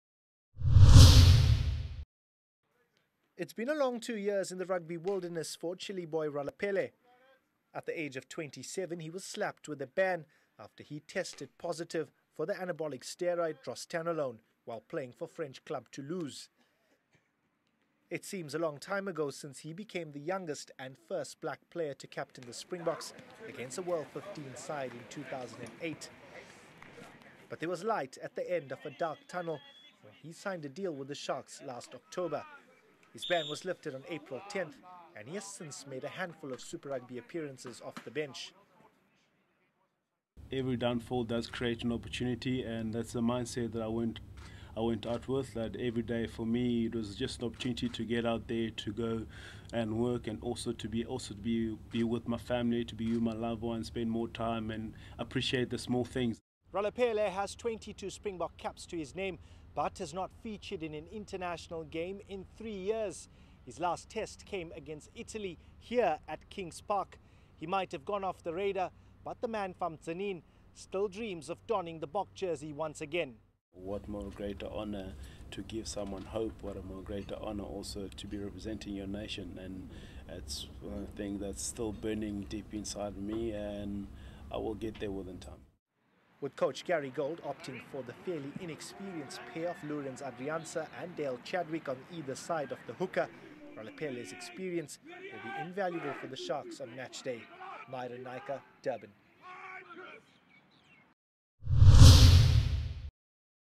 Chiliboy Ralepelle set to start first Super Rugby game in three years
JOHANNESBURG, 20 May 2016 - The Sharks hooker will start against the Southern Kings in Durban on Saturday, after returning from a two-year doping ban. His ban was lifted on April 10th and he has since made a handful of Super Rugby appearances off the bench.